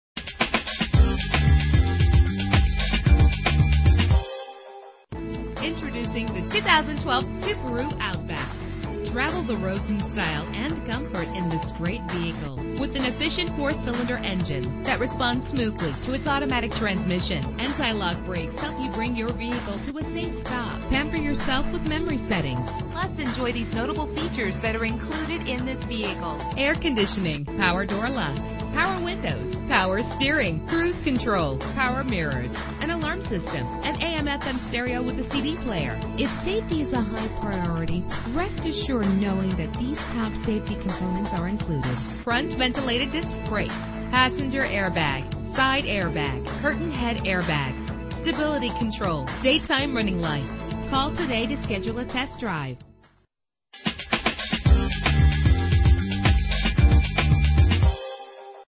2012 Subaru Outback - HOUSTON TX
Year: 2012
Make: Subaru
Model: Outback
Trim: 2.5i Premium
Engine: 2.5 liter H4 16 valve
Transmission: Automatic CVT
Color: Satin White Pearl
Mileage:
Address:
17109 KATY FREEWAY